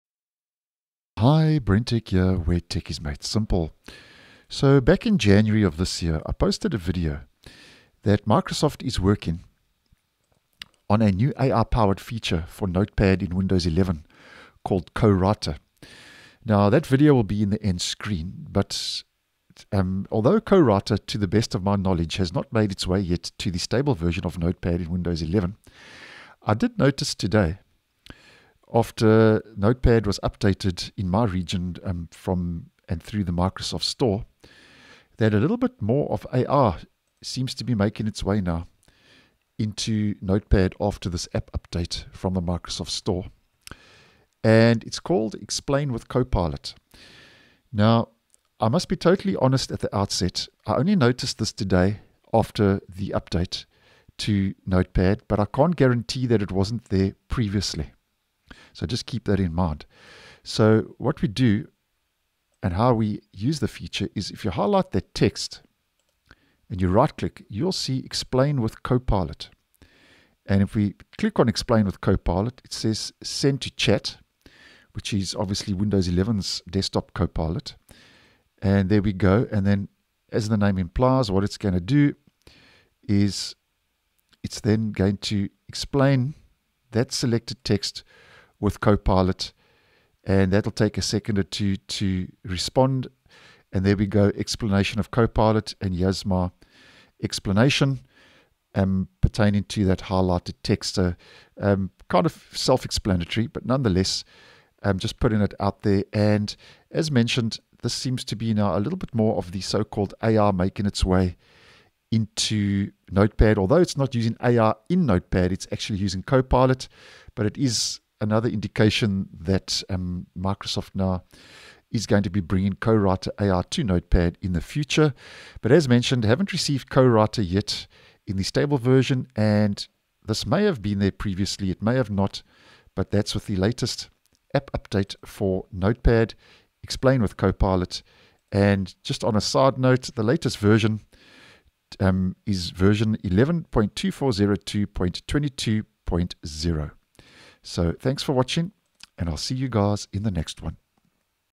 Windows 11 Notepad AI Features begin rolling out - New 'Explain with Copilot'
“Explain with Copilot”, where you can ask Copilot in Windows to help explain log files, code segments, or any selected content directly from within Notepad.